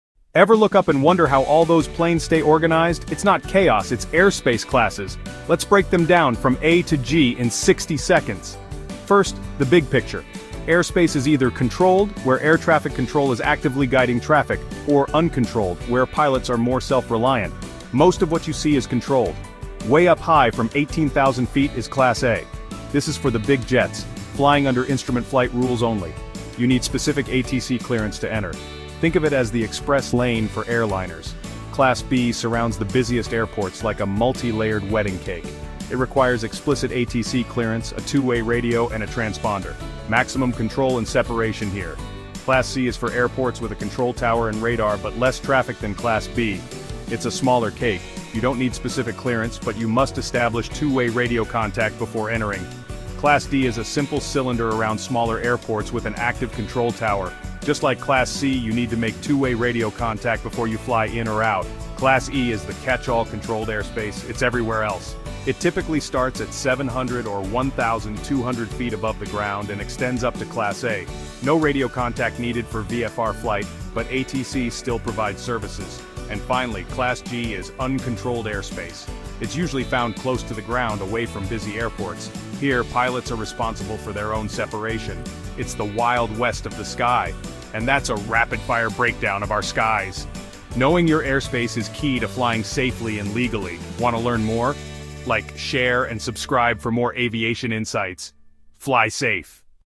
Understanding Airspace Classes – A‑G
Ever wonder how planes stay perfectly organized up in the sky? It’s all about Airspace Classes—from A to G! This quick, 1-minute breakdown explains controlled and uncontrolled airspace, from the high-flying Class A jets requiring strict ATC clearance, down to the free-roaming Class G where pilots navigate on their own. See the multi-layered “wedding cake” zones around busy airports, and learn why radio contact is a must in Classes B through D. Featuring stunning visuals: commercial airliners, Cessna aircraft, towering control towers, sectional charts, 3D airspace models, radar screens, and pilot headsets—all captured with crisp, cinematic detail and cool blue-teal tones. Perfect for student pilots and aviation enthusiasts eager to master the skies!

OUTLINE:

00:00:00 Introduction to Airspace Classes